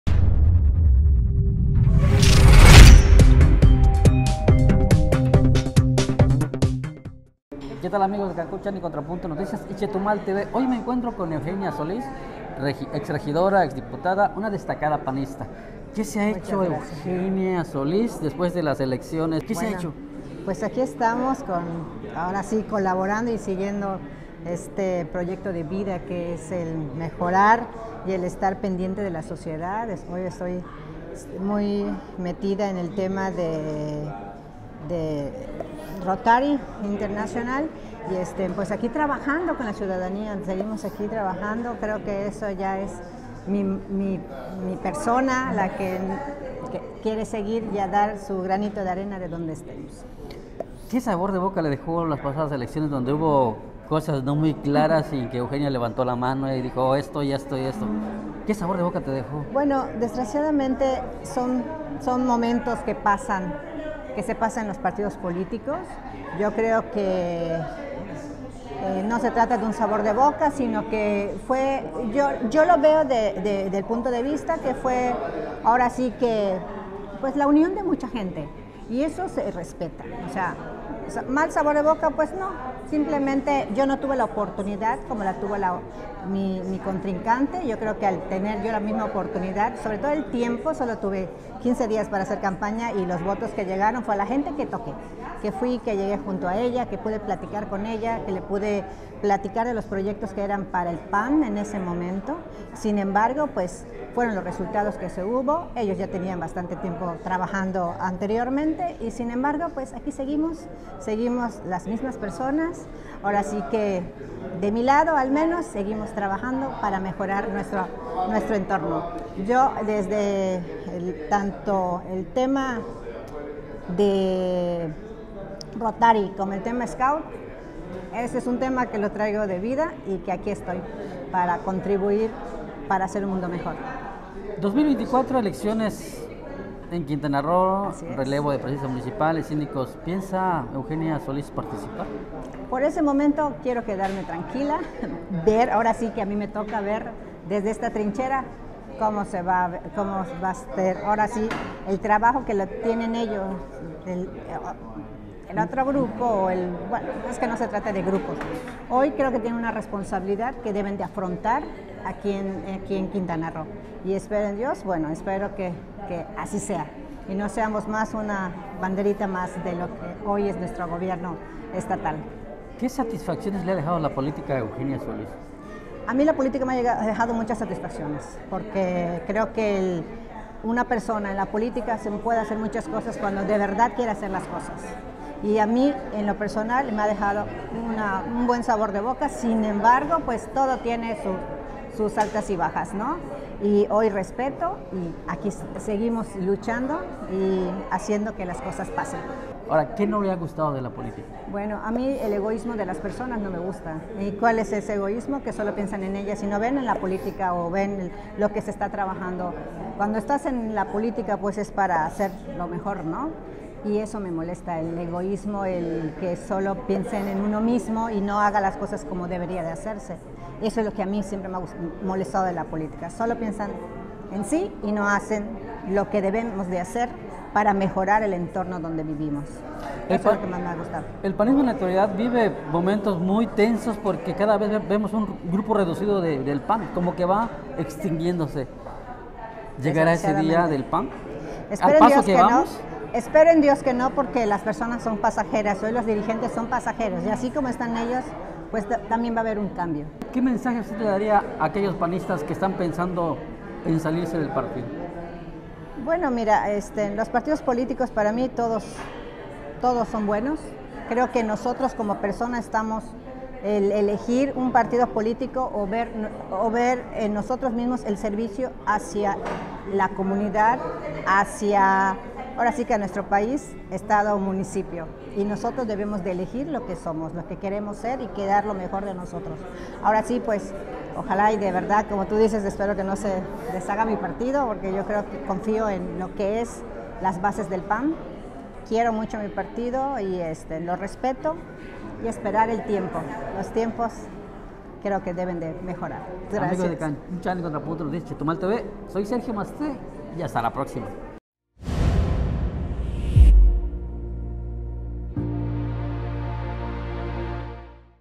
REVELA EUGENIA SOLÍS LO QUE NO LE GUSTA DE LA POLÍTICA
*La panista Eugenia Solís por ahora se dice tranquila y "me toca ver el trabajo" de los dirigentes del PAN en Quintana Roo, con miras a las elecciones del 2024